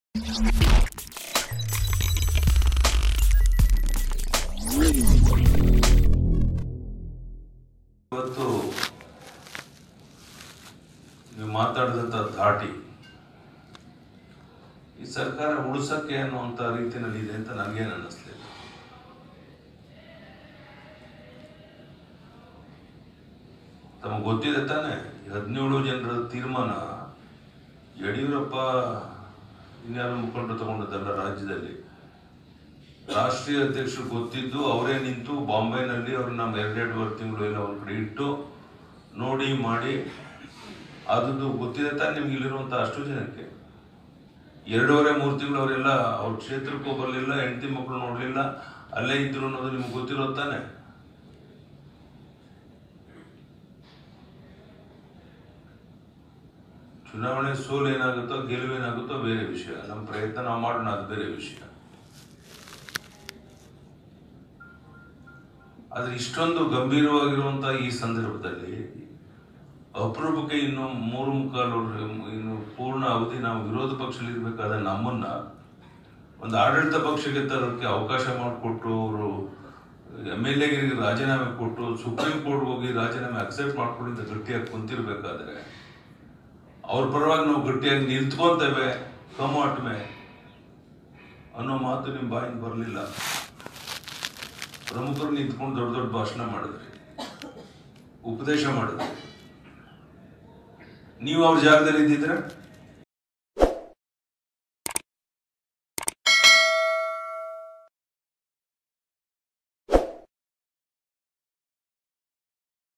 ಅನರ್ಹರು ಇಲ್ಲದಿದ್ದರೆ ಸರ್ಕಾರವೇ ಇರುತ್ತಿರಲಿಲ್ಲ, ಸಿಎಂ ಯಡಿಯೂರಪ್ಪ ಸ್ಪೋಟಕ ಆಡಿಯೋ ಬಹಿರಂಗ
ಹುಬ್ಬಳ್ಳಿ: ವಿರೋಧ ಪಕ್ಷದಲ್ಲಿದ್ದ ನಮ್ಮನ್ನು ಆಡಳಿತಕ್ಕೆ ತಂದಿದ್ದು ಅನರ್ಹ ಶಾಸಕರು. ಅವರ ಪರವಾಗಿ ನಾವು ಗಟ್ಟಿಯಾಗಿ ನಿಲ್ಲಬೇಕು ಎಂದು ಪಕ್ಷದ ಮುಖಂಡರಿಗೆ ಮುಖ್ಯಮಂತ್ರಿ ಬಿ.ಎಸ್.ಯಡಿಯೂರಪ್ಪ ಕಿವಿಮಾತು ಹೇಳಿದ್ದಾರೆ.

ಹುಬ್ಬಳ್ಳಿಯಲ್ಲಿ ಇತ್ತೀಚೆಗೆ ನಡೆದಿದ್ದ ಬಿಜೆಪಿ ಕೋರ್ ಕಮಿಟಿ ಸಭೆಯಲ್ಲಿ ಅನರ್ಹ ಶಾಸಕರ ಕ್ಷೇತ್ರದ ಉಪ ಚುನಾವಣೆ ಬಗ್ಗೆ ಚರ್ಚೆ ನಡೆದಿದೆ. ಈ ವೇಳೆ ಅನರ್ಹ ಶಾಸಕರ ವಿರುದ್ಧ ಪಕ್ಷದ ಮುಖಂಡರು ಮಾತನಾಡಿದ್ದಾರೆ. ಇದಕ್ಕೆ ಸಭೆಯಲ್ಲಿ ಅತೀವ ಬೇಸರ ವ್ಯಕ್ತಪಡಿಸಿದ ಬಿಎಸ್‍ವೈ ಬಿಜೆಪಿ ನಾಯಕರಿಗೆ ಫುಲ್ ಕ್ಲಾಸ್ ತೆಗೆದುಕೊಂಡಿದ್ದಾರೆ ಎಂದು ಮೂಲಗಳಿಂದ ತಿಳಿದು ಬಂದಿದೆ.